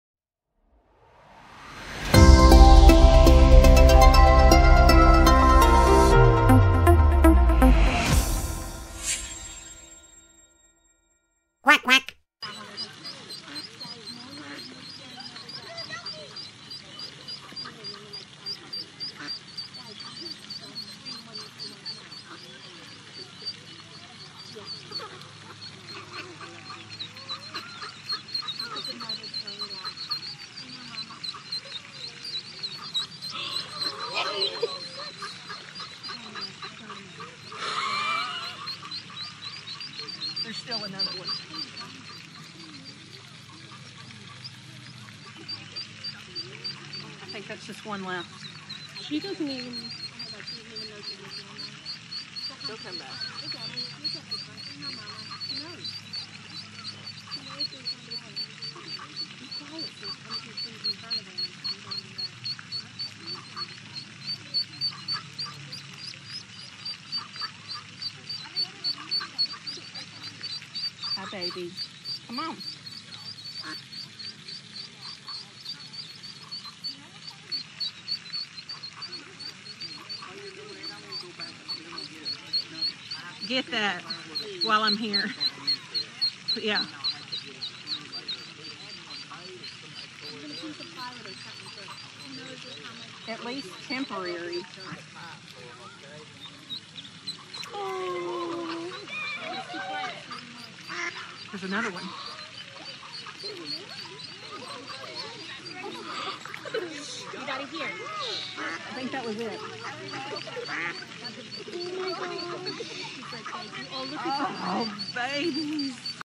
10 Baby Ducklings Reunited with Momma and Sister
While walking past the pond with their classes, preschool teachers Mrs. Julie and Ms. Laruen saw one mother duck and one baby duck. It was strange to only see one baby with its momma. On their return trip back by the pond, they noticed the mother and baby swimming close to an overflow drain, and then they heard the chirp-like peeps of the babies that had fallen into the drain. Mrs. Julie quickly took off her shoes and waded into the pond, jeans and all, to rescue the 10 trapped baby ducks!